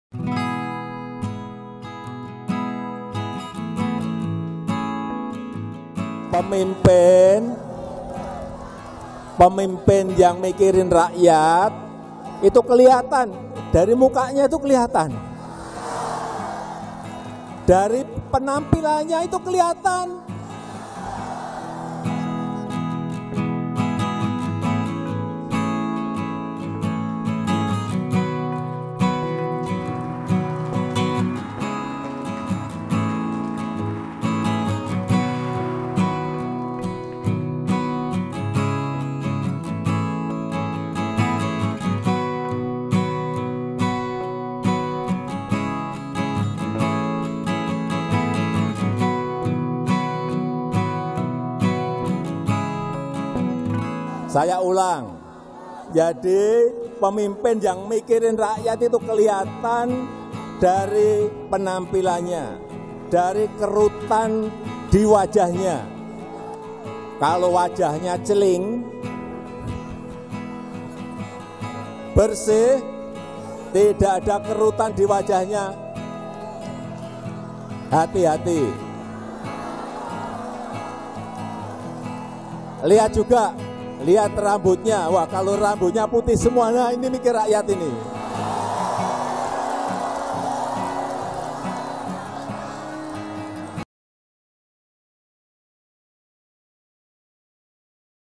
Respons Pernyataan Jokowi, Ganjar Pranowo Unggah Foto Berambut Hitam dan Wajah Tanpa Kerutan
JAKARTA, KOMPAS.TV - Pernyataan Presiden Joko Widodo (Jokowi) soal ciri pemimpin yang pro rakyat itu berambut putih dan wajah ada kerutan jadi sorotan publik.

Banyak pihak yang mengaitkan sosok tersebut dengan Gubernur Jawa Tengah Ganjar Pranowo.

Lantaran secara penampilan Ganjar dinilai memenuhi kriteria fisik yang disebut Jokowi.

Namun, baru-baru ini, Ganjar Pranowo justru merespons dengan mengunggah foto dirinya di akun Instagram resmi miliknya dengan gaya berambut hitam dan berwajah tanpa kerutan.

"Cukur, kamu punya tips merawat wajah & rambut?" Tulis Ganjar, pada Minggu (27/11/2022).

Baca Juga Jokowi Sebut Ciri Pemimpin Pro Rakyat "Rambutnya Putih", Kode Dukung Ganjar Pranowo? di

Netizen pun ramai-ramai merespons unggahan Ganjar tersebut.

"Lho pakkudah masuk kriteria jangan disemir hitam." Balas akun you_meymey.

"Waktu belum memikirkan rakyat ya pak?" Tulis akun amh_story.

Diktehui, sebelumnya Jokowi ungkap ciri pemimpin pro rakyat di hadapan ribuan relawan, pada Sabtu (26/11/2022).

Menurutnya, ciri pemimpin yang memikirkan rakyat terlihat dari penampilan fisiknya, salah satunya memiliki kerutan di wajah dan rambutnya putih semua.